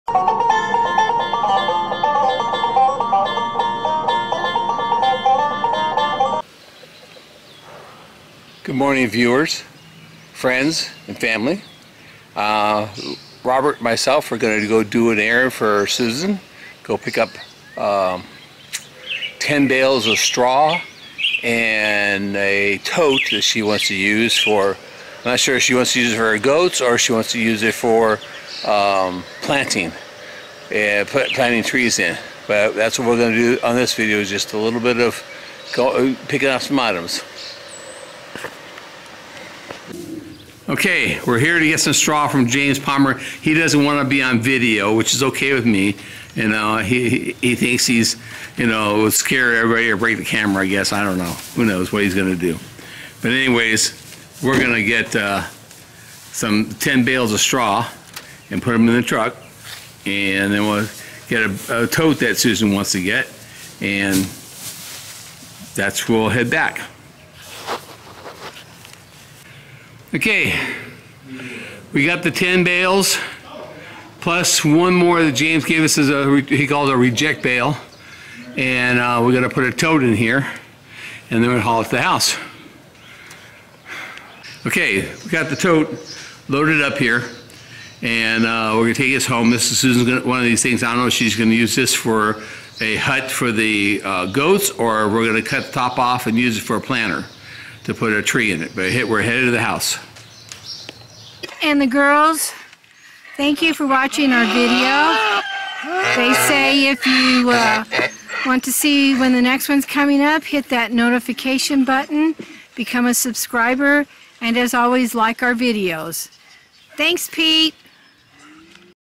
Straw bales for dog pens
got some straw bales to put in the dog pens.